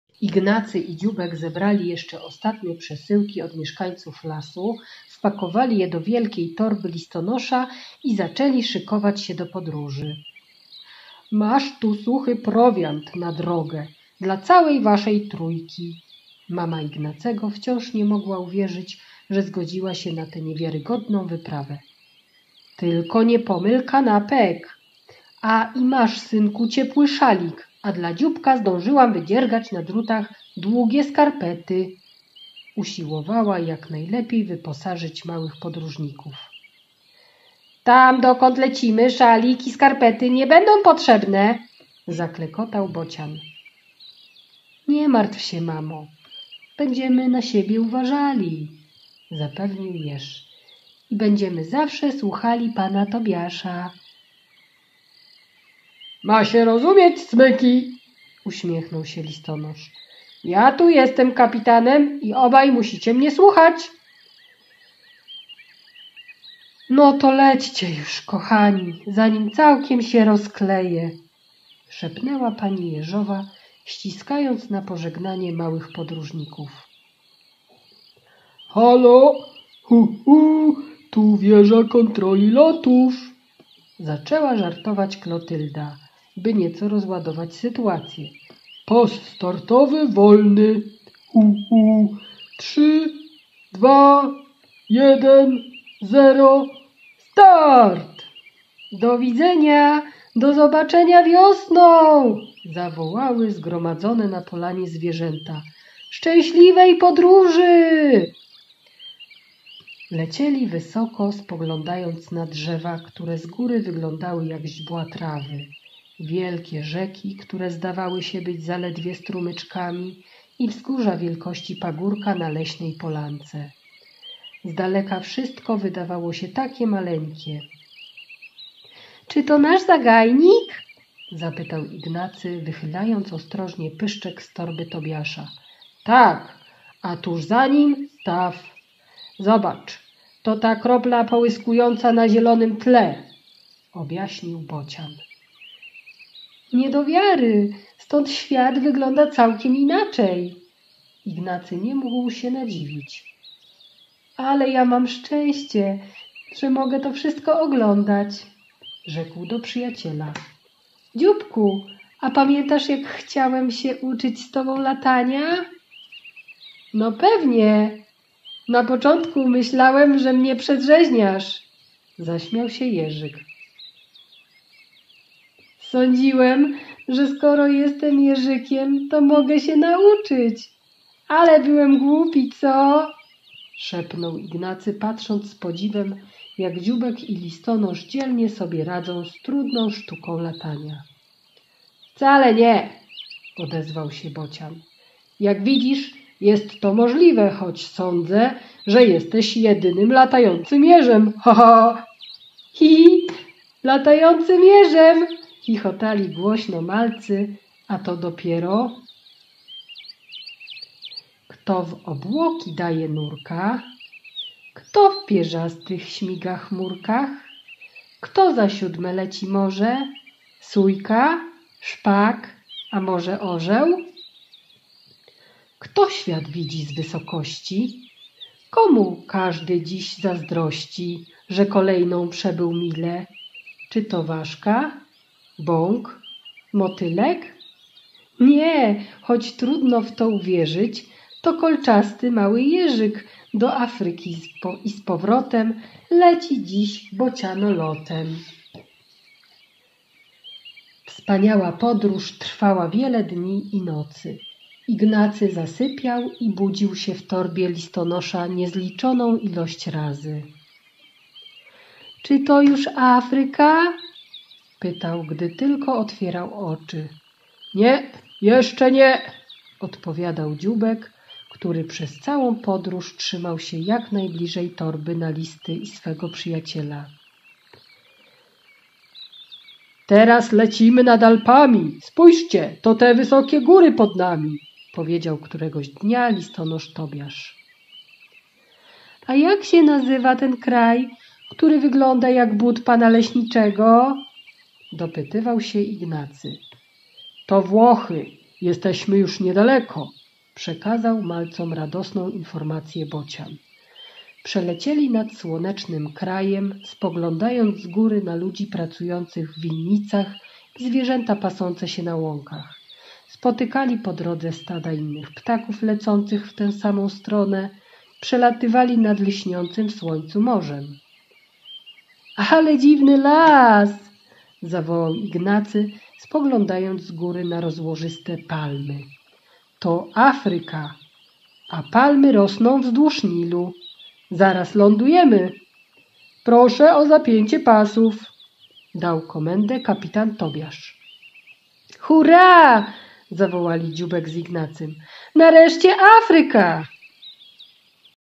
Czytanka dla Malucha - OKwM - Jak jeżyk z jerzykiem odkryli Afrykę/U. Kozłowska, rozdz. 10
Klub Malucha, czyta: Monika Deptuła